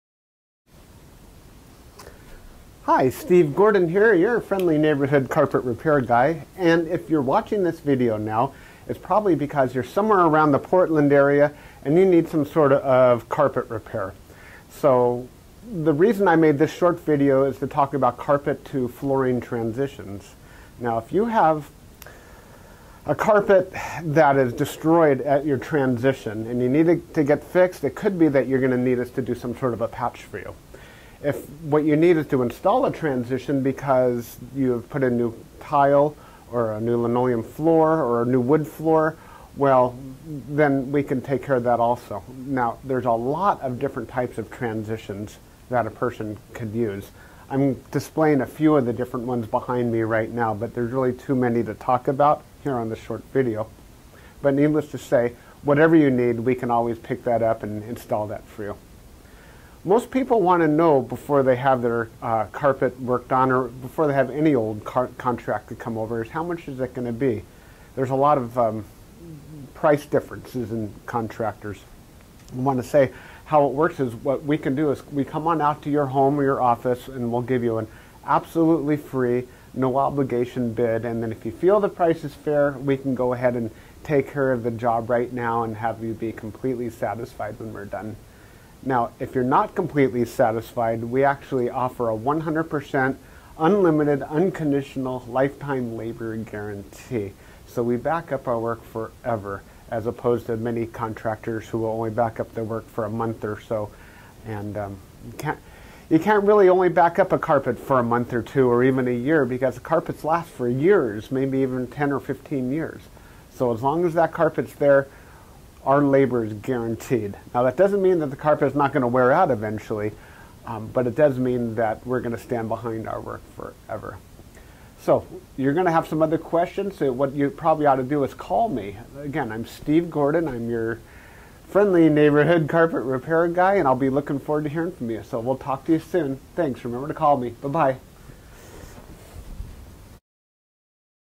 Portland Carpet To Floor Transitions
If your carpet has more waves than the Pacific ocean you may need to have your carpet stretched. If that's you, We can help and then we back up our Portland carpet repair with a full 100% Unlimited, Unconditional Lifetime Labor Guarantee.